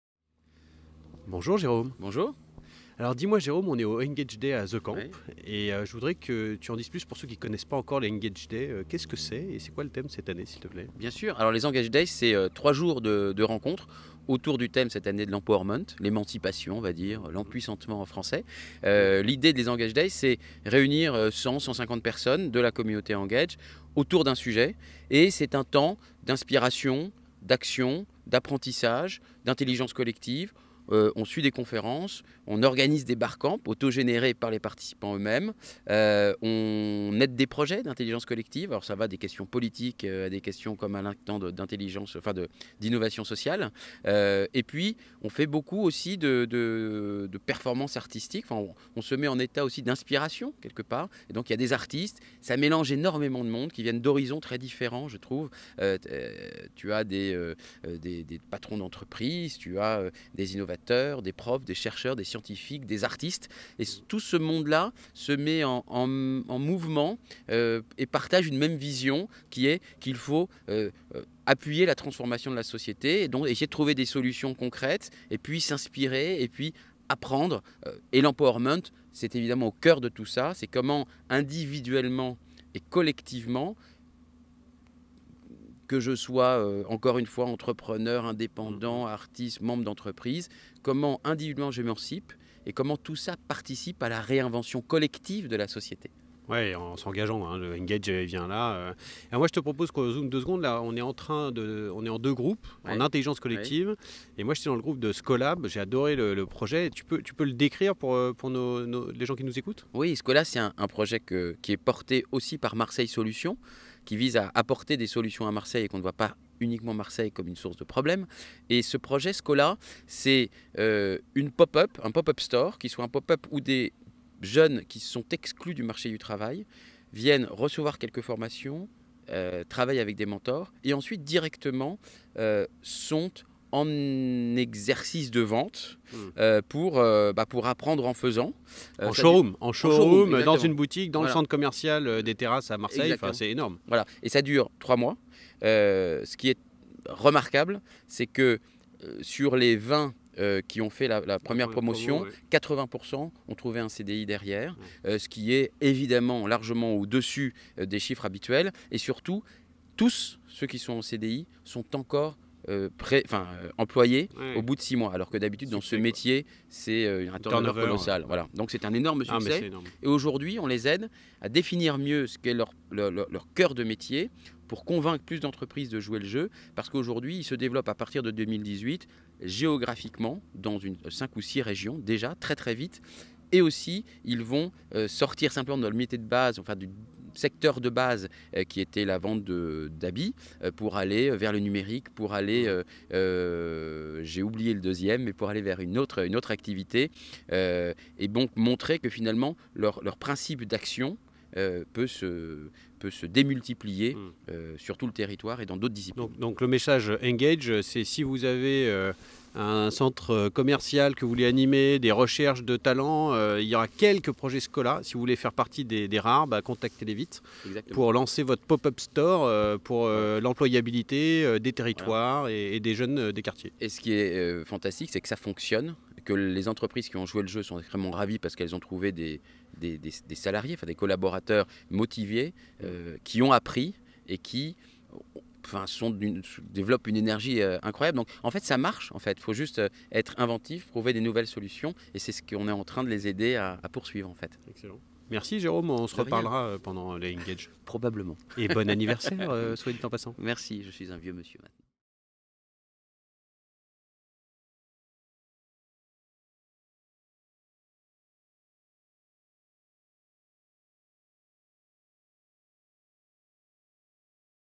Interview de Jerome Cohen par Julien Carlier
Pendant les EngageDays à The Camp, Julien Carlier a interviewé Jérôme Cohen - fondateur de Engage.
Concept et thème 2017 "Empowerment" ainsi qu'intelligence collective et reflexions autour du projet Skola...